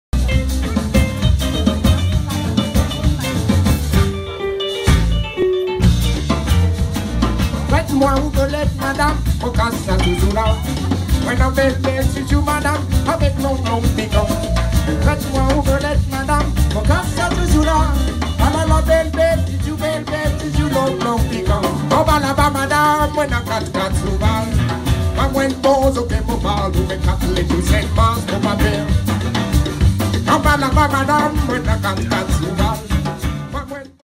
Les nuits de la poterie 2019 - Les Pythons de la fournaise
Concert du 10 août 2019 à Lhuis
Les Pythons de la fournaise

Clémentine Duchemann (chant / kayamb / claves)
Félix Vincent (chant / kayamb / percussions)
Mathieu Félix (guitare / clavier)
David Grasseli (guitare / choeurs)
Jean-Philippe Curtelin (batterie)
Lucas Terito (basse)